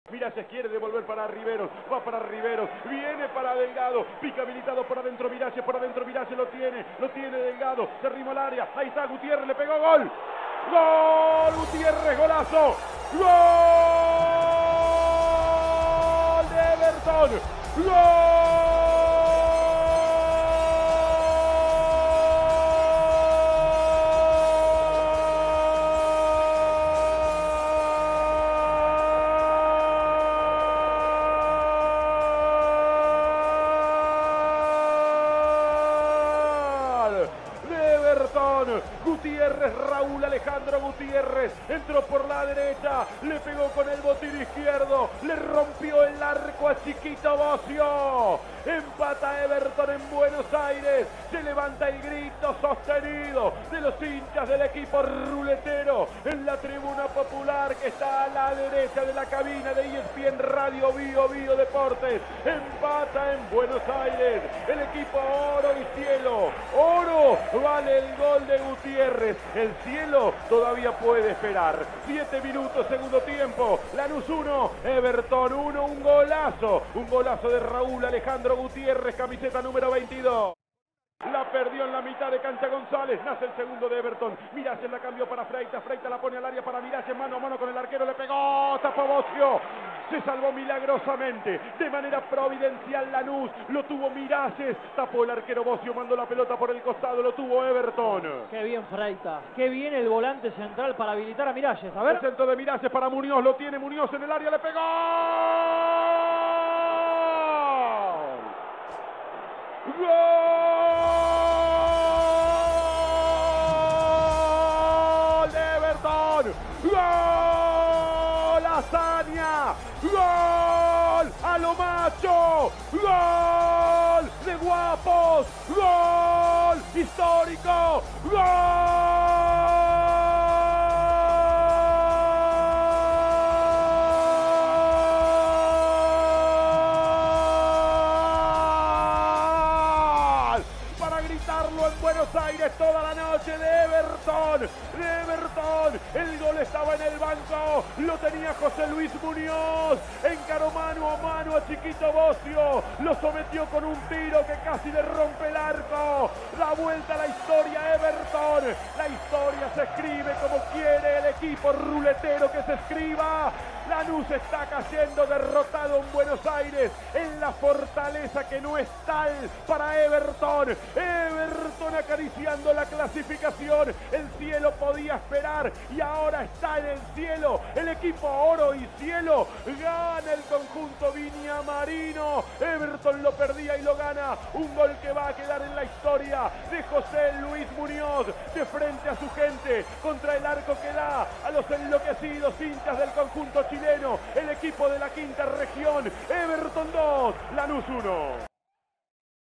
A 14 años de la hazaña ! Un relato épico para un partido histórico Lanus 1 Everton 2
Ya son 14 años de la gesta histórica que tuvo Everton al ser el primer equipo chileno en ganar en tierras argentinas por Copa Libertadores de America. Un recuerdo imborrable para los viajeros y para toda la familia Evertoniana!